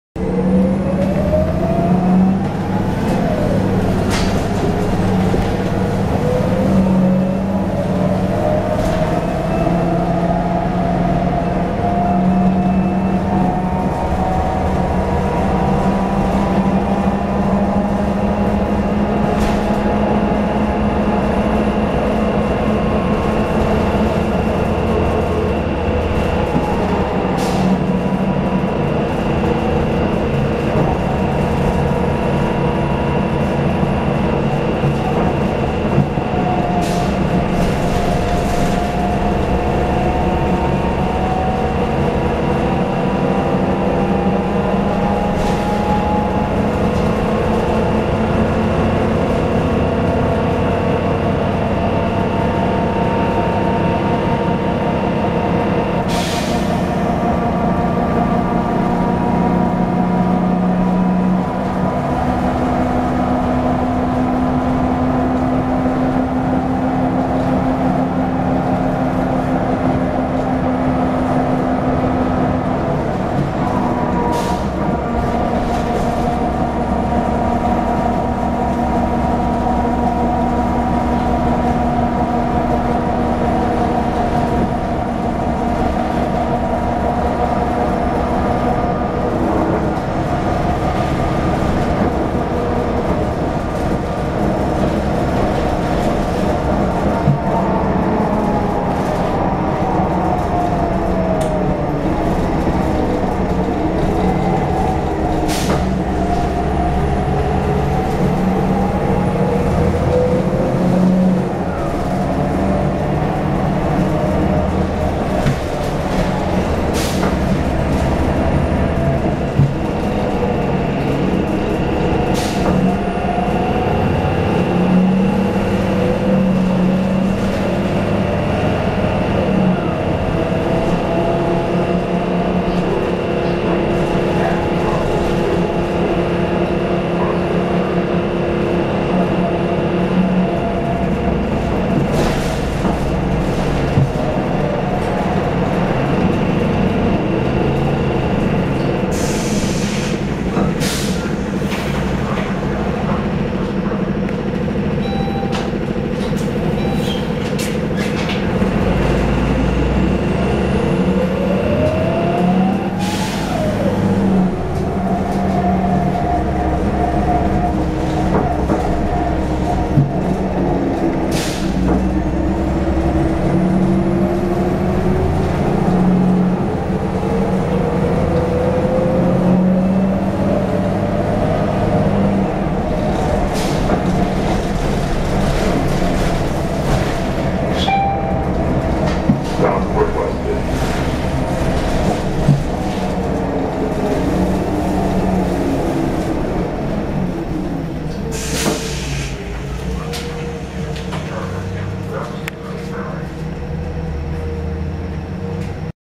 MBTA #2041 (The Ride)
On the 52 line to Watertown Yard.

This is a 2003 NABI 40LFW CNG with a Cummins C Gas+ engine and Allison B400R transmission.